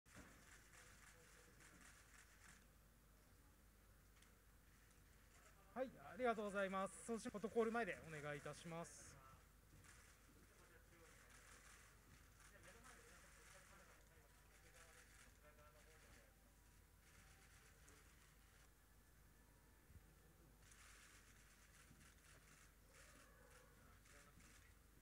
矢野未希子 大胆美背中 Mikiko Yano - "モエ・エ・シャンドン EFFERVESCENCE シャンパンの魔法と輝きを" Photo Call
矢野未希子が８日、東京タワー地上特設会場で行われた『モエ・エ・シャンドン“EFFERVESCENCE – エフェルヴェソンス”シャンパンの魔法と輝きを』オープニングセレモニーに出席。フォトコールに臨んだ。